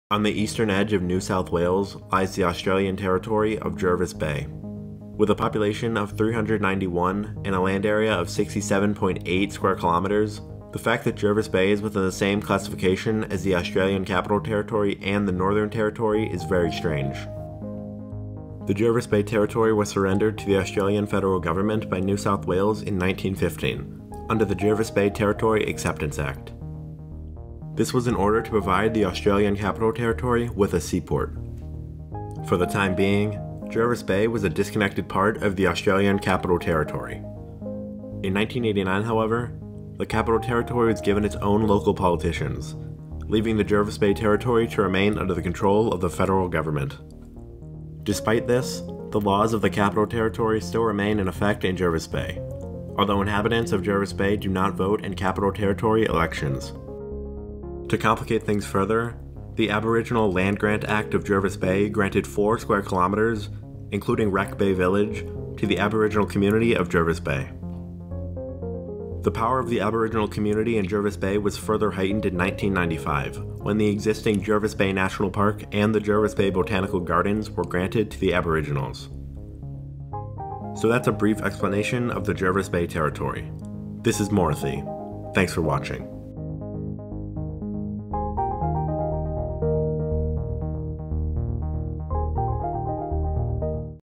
Australia's Jervis Bay Territory Explained
In this video, I briefly explain the strange case of Australia's Jervis Bay Territory.